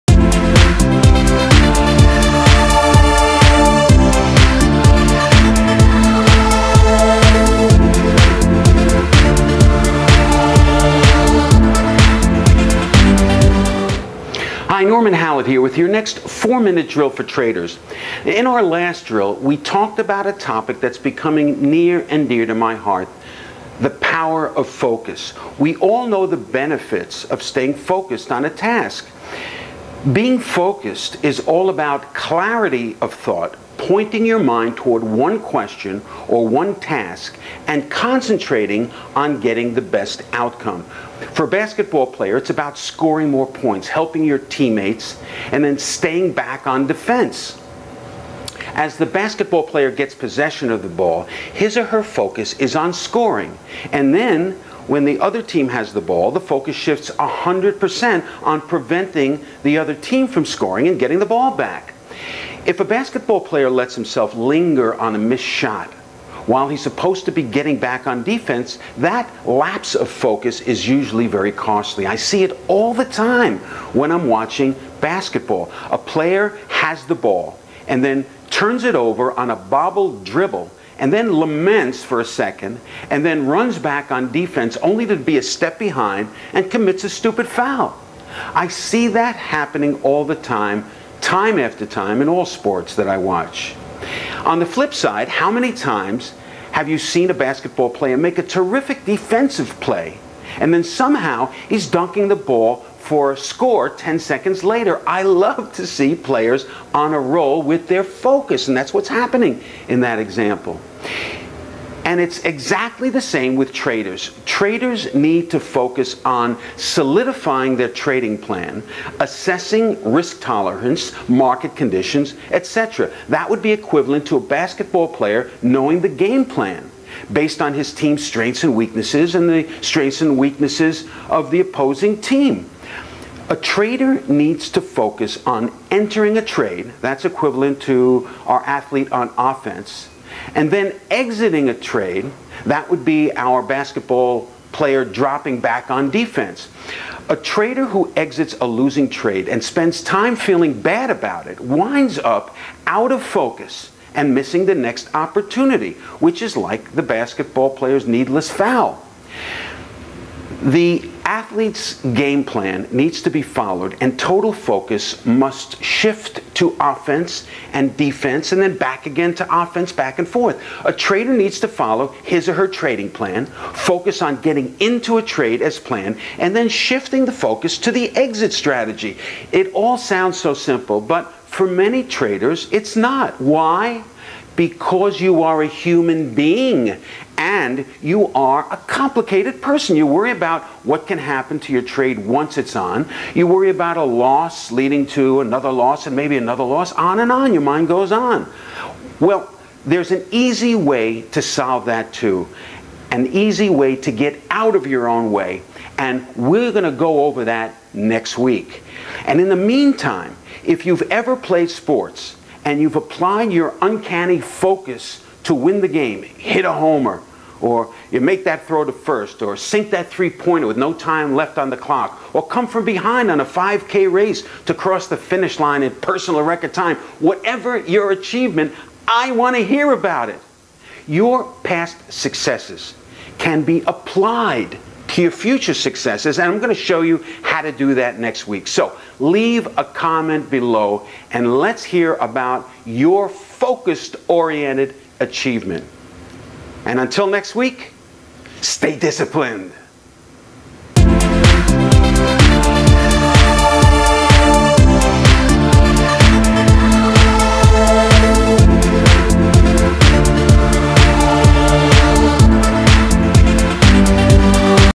Importance of Focus Part 2 - Week 102 - 4-Minute Drill for Traders
This week, Norman uses a sports analogy to help explain the power of focus and how it can effect our "game."  The trader needs to follow his or her trading plan... focus on getting into a trade as planned and then shifting focus to the exit strategy.  It sounds simple, but, for many traders, it's not.  Norman explains why in
this week's 4-Minute Drill.